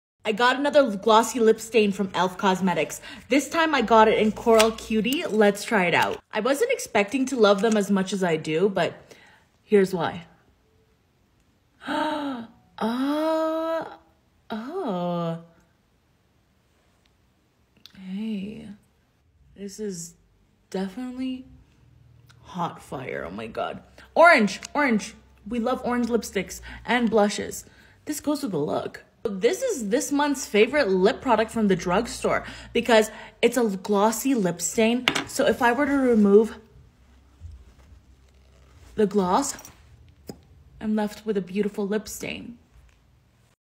ANOTHER ONE!! ELF GLOSSY LIP STAIN ARE 🔥
I told y'all I'd get more shades 😊

Products Mentioned in this video:

E.L.F Cosmetics - Glossy Lip Stain in Coral Cutie

T I K T O K :  GLAMZILLA
I N S T A G R A M : GLAMZILLA
P I N T E R E S T : GLAMZILLA